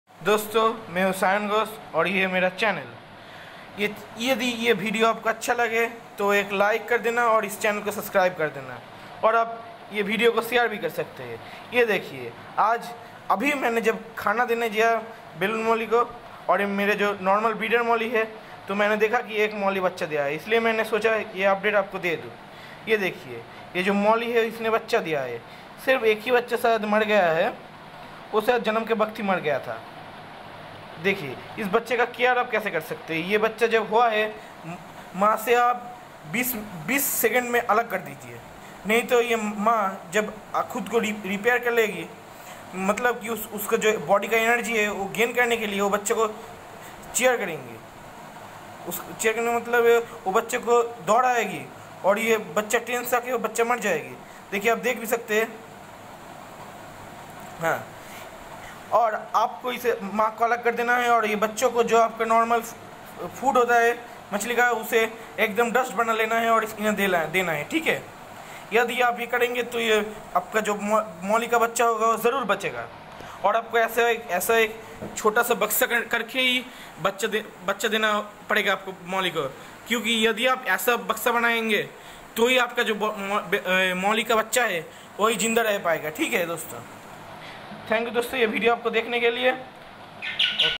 My Black Molly fish giving birth to his young babies
How to care and breed a Black Molly fish in a small plastic box